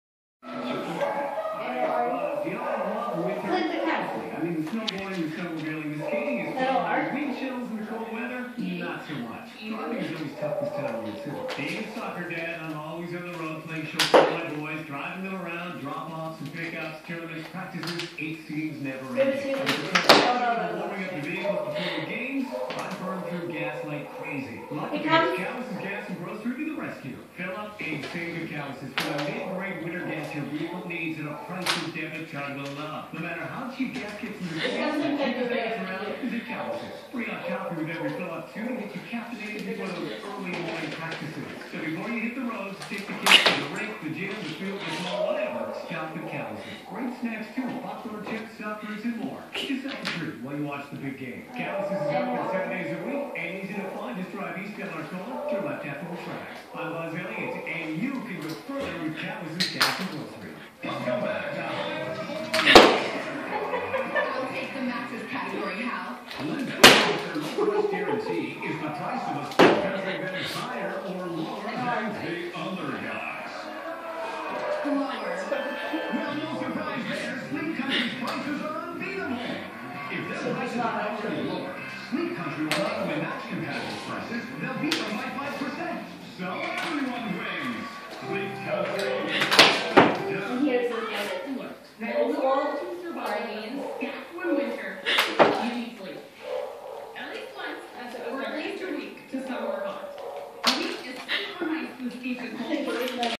Sola playing with markers
Sola had a ball trying to run away with tied down markers lol